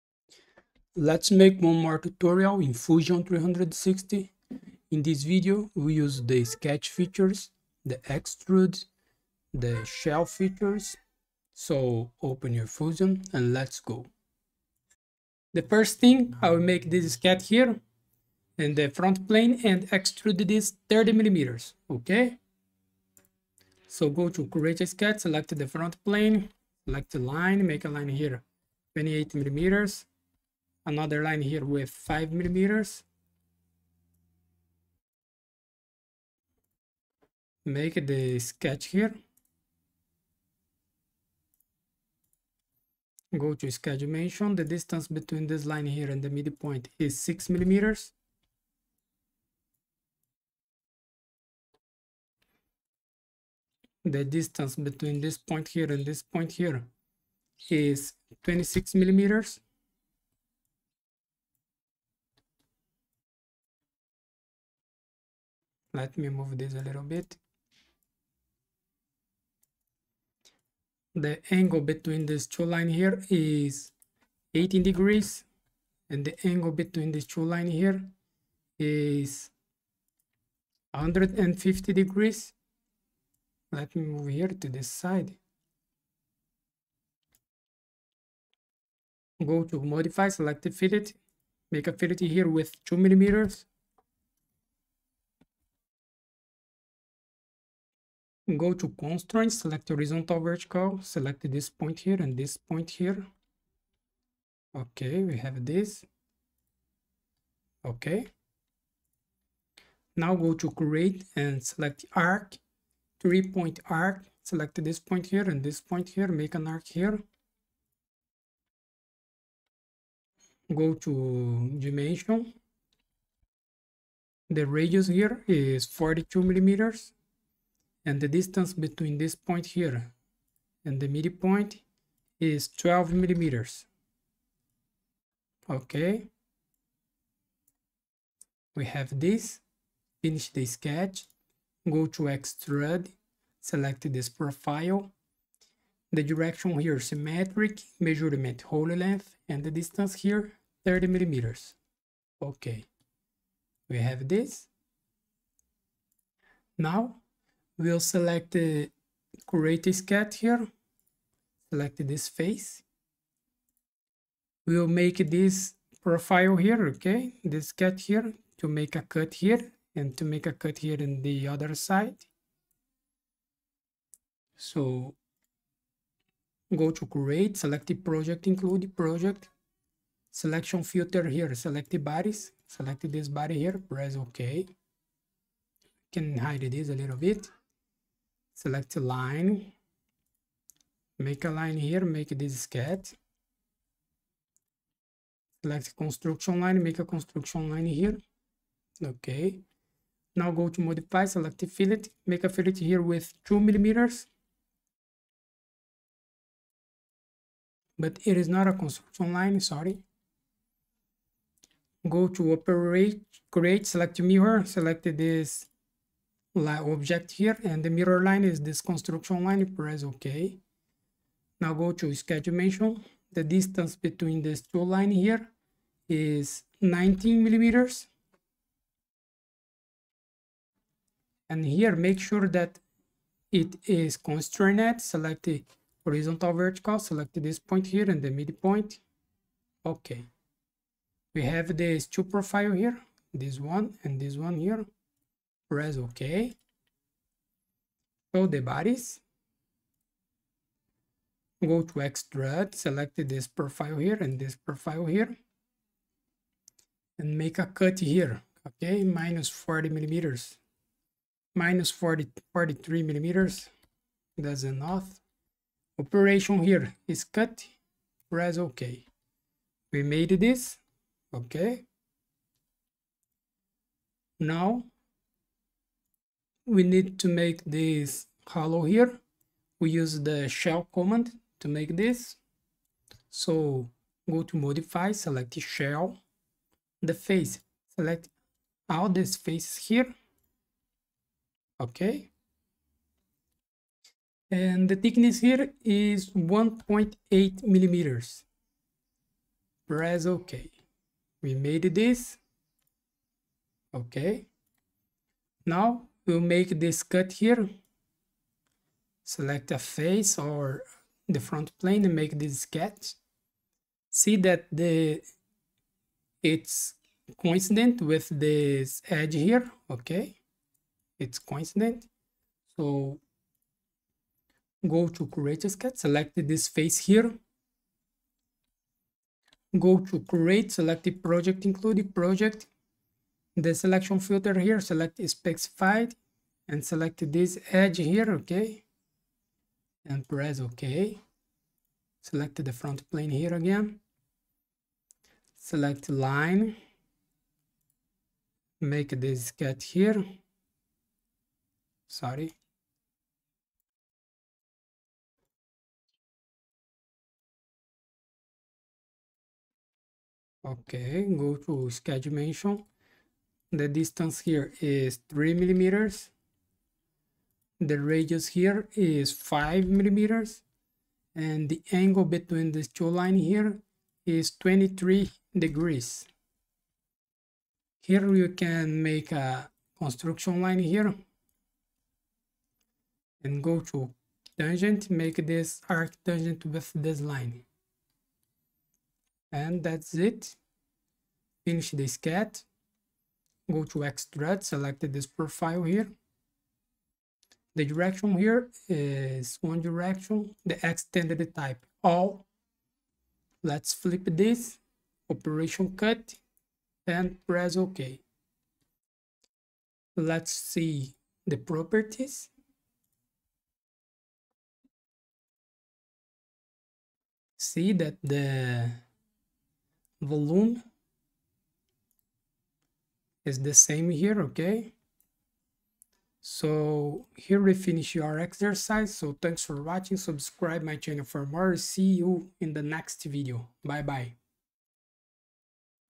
How to use Shell Feature in Fusion 360 - Exercise 279
In this tutorial we'll use the features:
Extrude
Shell